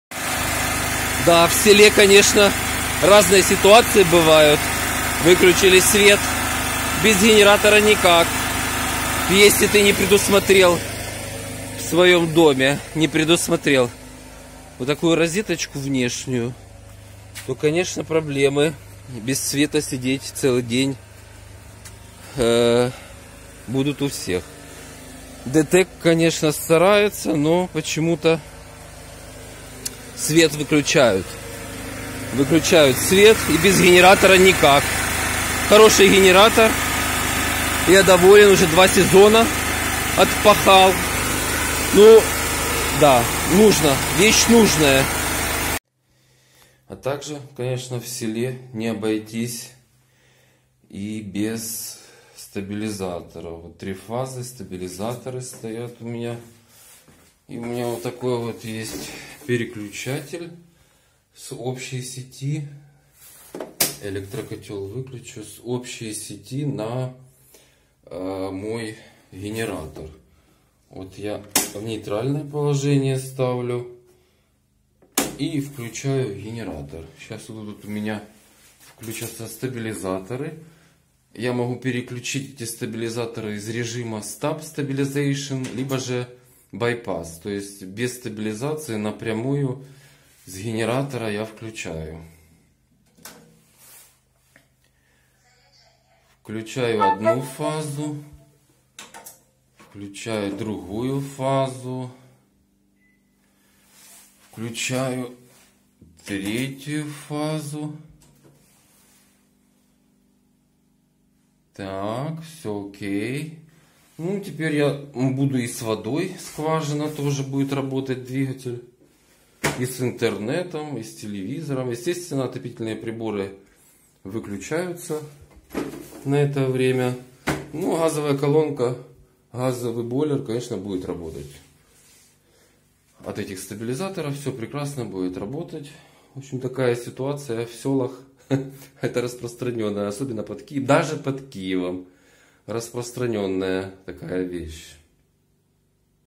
Пропал свет. Включил бензогенератор Forte FG3800. Киевская область. Київщина. Хата в селі
Про життя в селі на Київщині